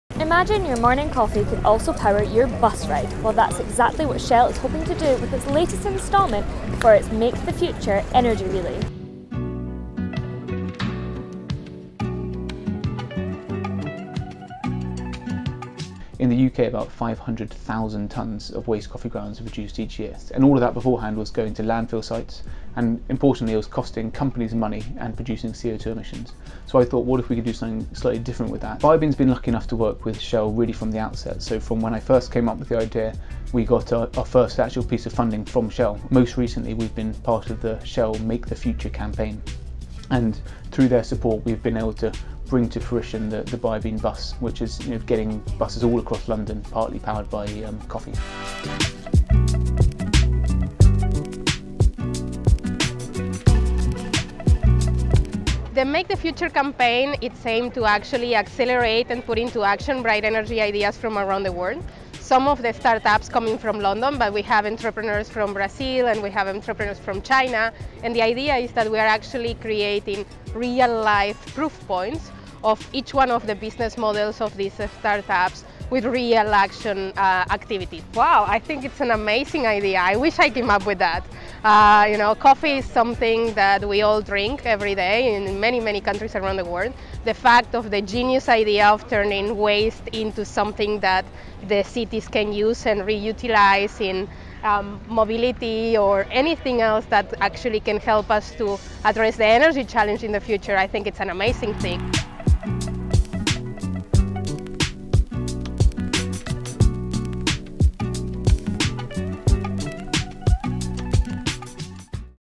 London’s Buses Could Now Be Fueled By Your Morning Coffee
Shell is hoping to fuel your morning bus ride using leftover coffee grounds as it invests in a new bio feul created by Bio-bean.

With over 5000 tonnes of waste coffee grounds being produced by the UK each year, Bio-bean founder, Arthur Kay, made it his mission to reduce this, thus creating the bio-bus. A bus solely fuelled by the coffee grounds oil and diesel combination.

Kay said: “Bio-bean has worked with Shell from the outset from when I first came up with the idea. We got the first actual piece of funding from them and through their support we’ve been able to bring to fruition the bio-bean bus.”

This is the latest instalment in Shell’s MakeTheFuture global energy relay, which sees six entrepreneurs come together to create innovative and eco-friendly alternatives to daily tasks. One has included the ‘Jump For Your Lunch’ campaign that saw people pay for their kinetic energy.

Malena Cutuli, group head of integrated brand communications and capability, created and developed the initiative and has been outspoken about her support of it all. She said: “I wish I had come up with the idea.

“Coffee is something that we all drink in many countries around the world, the way that they [Bio-bean] are turning waste into something that cities can re-use and re-utilise, whether that mobility or anything else that can help us address the energy challenge in the future, is an amazing thing.

“A good idea can come from anywhere, but with the scale and ambition of Shell, we can together make true progress.”